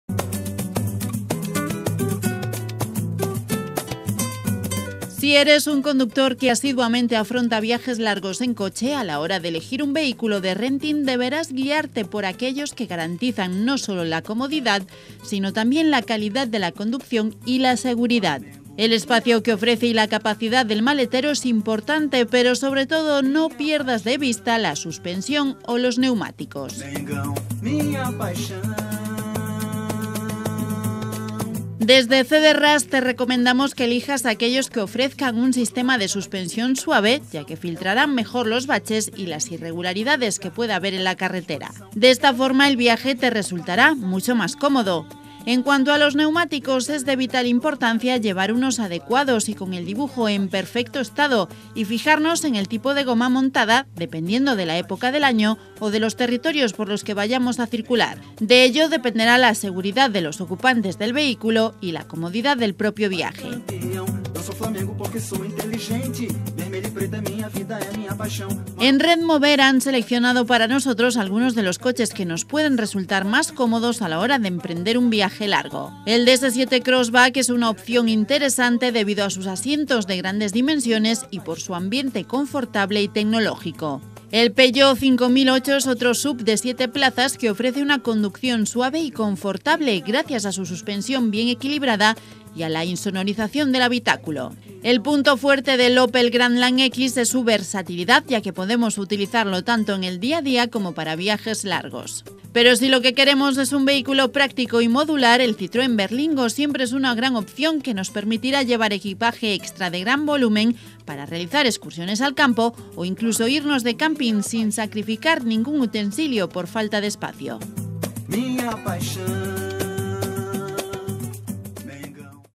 RENTING-COCHES PARA VIAJAR | By CdRas
Si eres un conductor que asiduamente afronta viajes largos en coche, a la hora de elegir un vehículo de renting deberás guiarte por aquellos que garantizan, no solo la comodidad, sino también la calidad de la conducción y la seguridad. El espacio que ofrece y la capacidad del maletero es importante pero, sobre todo, no pierdas de vista la suspensión o los neumáticos.

Desde CdRas te recomendamos que elijas aquellos que ofrezcan un sistema de suspensión suave ya que filtrarán mejor los baches y las irregularidades que pueda haber en la carretera. De esta forma el viaje te resultará mucho más cómodo. En cuanto a los neumáticos es de vital importancia llevar unos adecuados y con el dibujo en perfecto estado, y fijarnos en el tipo de goma montada dependiendo de la época del año o los territorios por los que vayamos a circular. De ello dependerá la seguridad de los ocupantes del vehículo, así como la comodidad del propio viaje.

En Redmover han seleccionado para nosotros algunos de los coches que nos pueden resultar más cómodos a la hora de emprender un viaje largo. El DS 7 Crossback es una opción interesante debido a sus asientos de grandes dimensiones y por su ambiente confortable y tecnológico. El Peugeot 5008 es otro SUV de siete plazas que ofrece una conducción suave y confortable gracias a su suspensión bien equilibrada y a la insonorización del habitáculo. El punto fuerte del Opel Grandland X es su versatilidad ya que podemos utilizarlo tanto en el día a día como para largos viajes. Pero si lo que queremos es un vehículo práctico y modular, el Citröen Berlingo siempre es una gran opción que nos permitirá llevar equipaje extra de gran volumen para realizar excursiones al campo o incluso irnos de camping sin sacrificar ningún utensilio por falta de espacio.